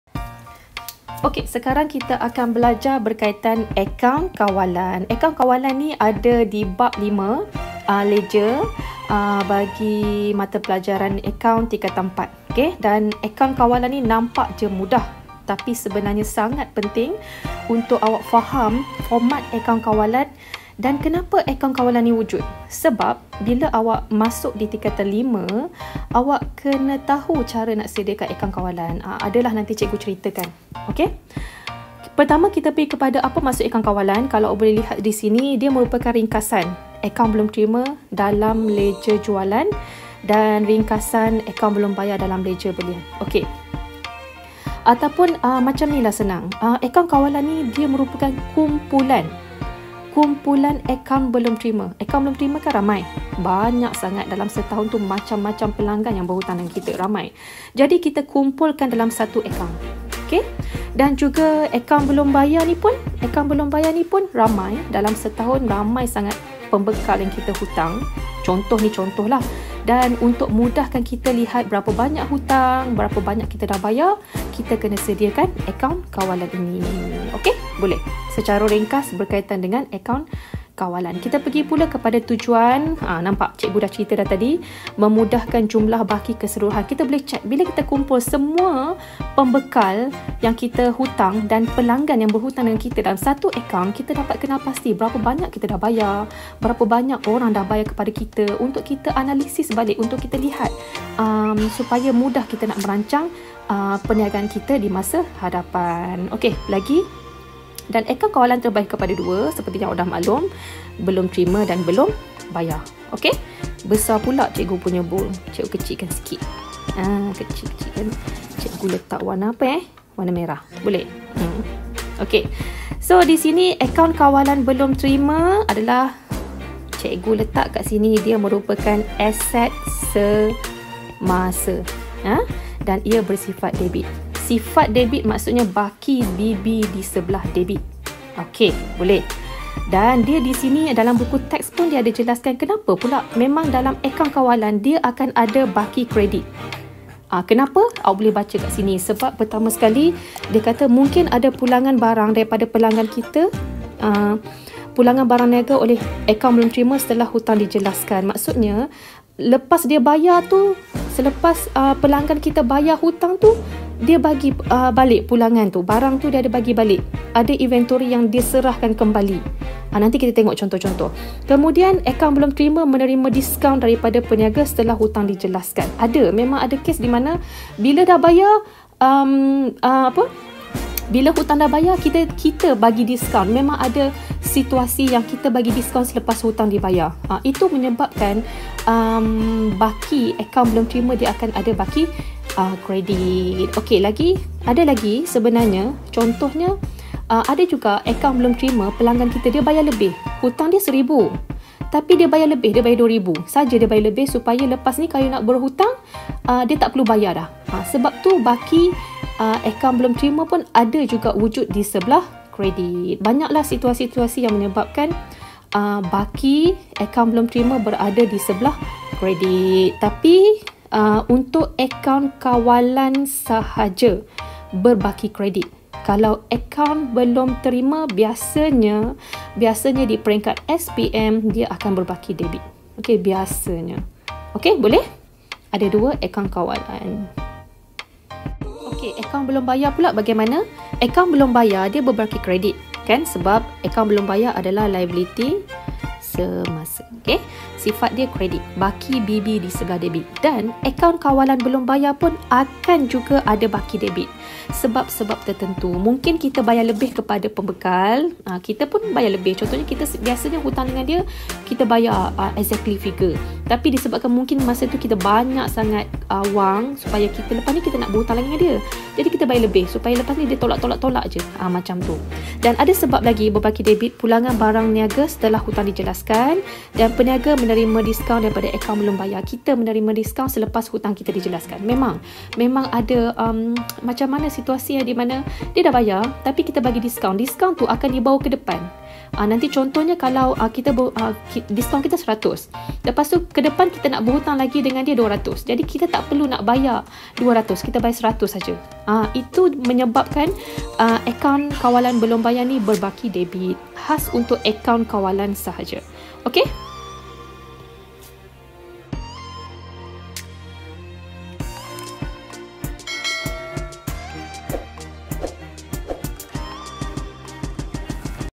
Bil 01 Pengenalan Akaun Kawalan Prinsip Perakaunan Tingkatan 4
Dapatkan nota R&R Tingkatan 4 khas cikgu buatkan untuk awak semua.. untuk tempahan boleh click sites cikgu di atas. Akaun tu mudah laaaaaaa